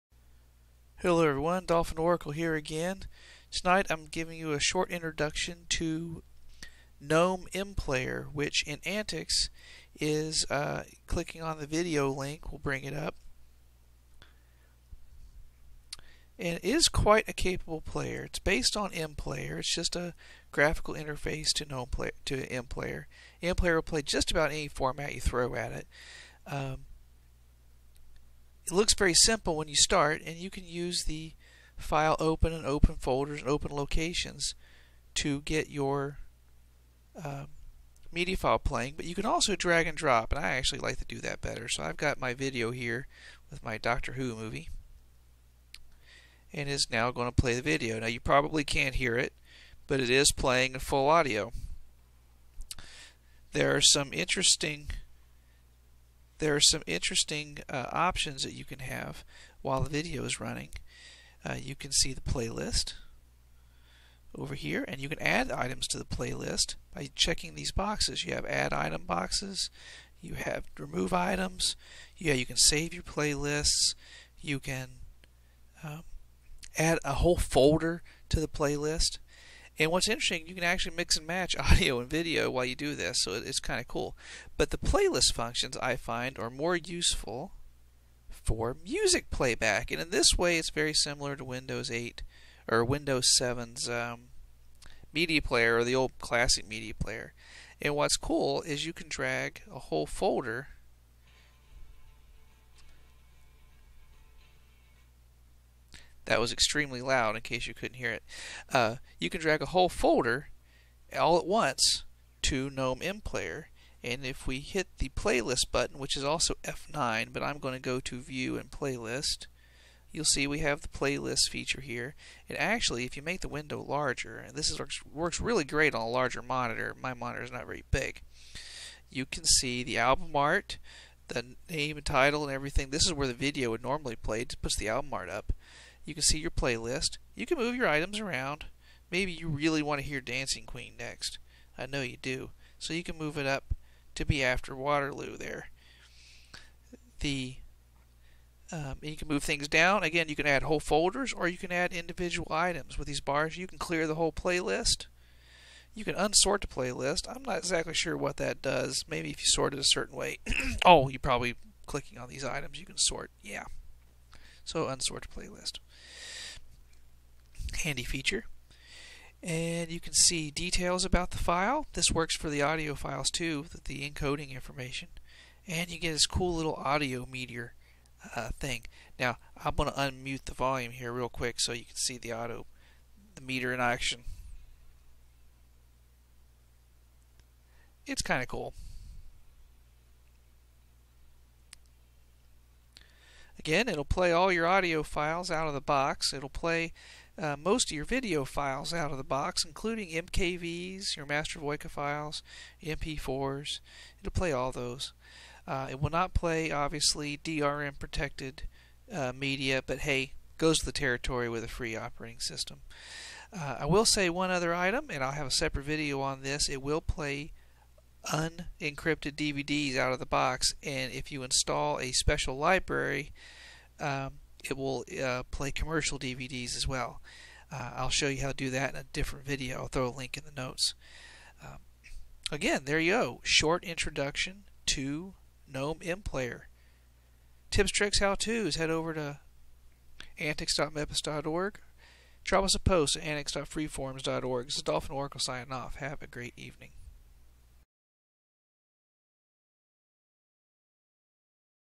AntiX 13 Stable -Gnome-MPlayer - play all your media files!
Here's a brief into to gnome-mplayer, showing off its ability to play both video and audio media files, its playlist features, and some other cool bits.  Runs great on older hardware.

If you install libdvdcss2 (see my video you can play your commercial dvd's as well.

d.o.